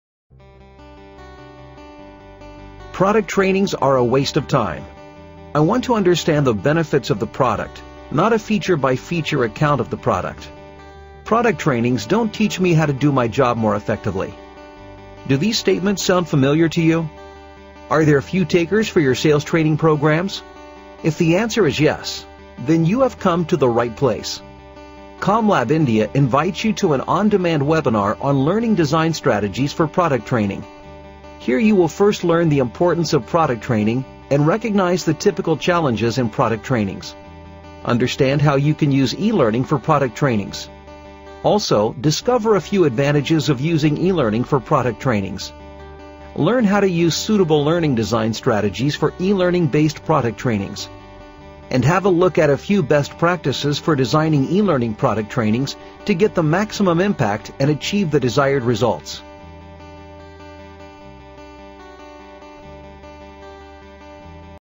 How to develop Strategies for Effective Product Training?
Are you excited to Learn the importance and advantages of using suitable learning design techniques and best practices to enhance your product training programs? Join us to discover practical tips and techniques to design engaging and impactful training sessions.

Find out how to use appropriate design strategies for feature-based product trainings.